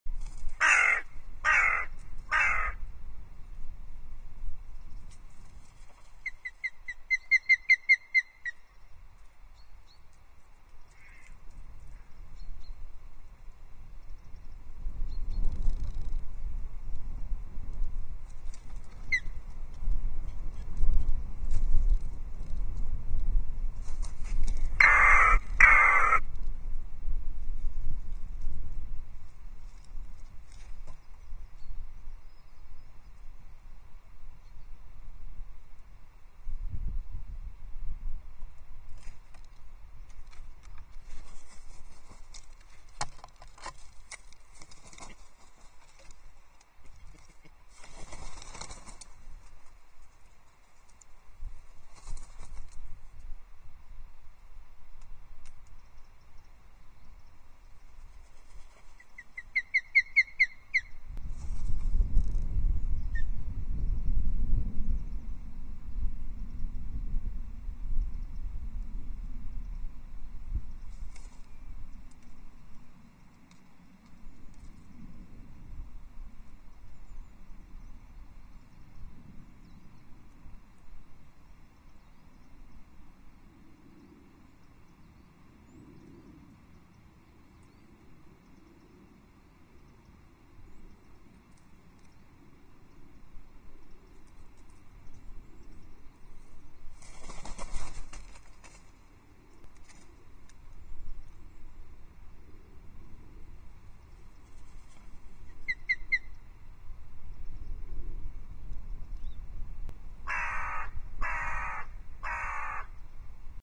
Dorcha has problems with Hooded Crows invading the Loch Arkaig Osprey Nest 23 Jul 2023
06.15 to 06.39
- A crow lands on or next to the camera - 6:15:37 - Dorcha glides up and lands on the perch, I’m assuming she’s warning the crow off but it’s not frightened off and at 6:16:13 - the crow flits down onto the nest, shortly joined by a second crow briefly touches down then flies off… 6:16:19 - the first crow then flies off but not before cheekily taking a twig with it.
Dorcha arrives 06.38 then the pesky crow climbs in the nest but swiftly chased off by Dorcha who returns almost immediately